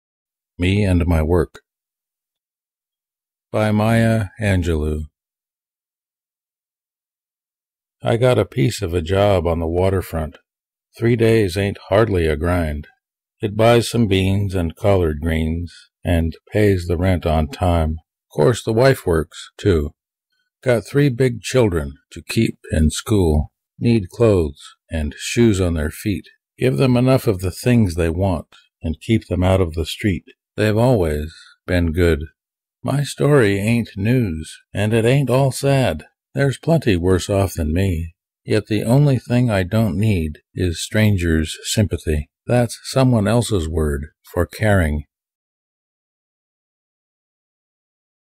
"My Work and Me," by Maya Angelou
poem--from I Shall Not Be Moved, by Maya Angelou (1991, Bantam) copyright Maya Angelou, grateful acknowledgement. Photo of Angelou from mayaangelou.com, photo of docks from theviewfromhere.com (copyright), photo of beans, collard greens, and cornbread from mothersmealplan.com, grateful acknowledgement.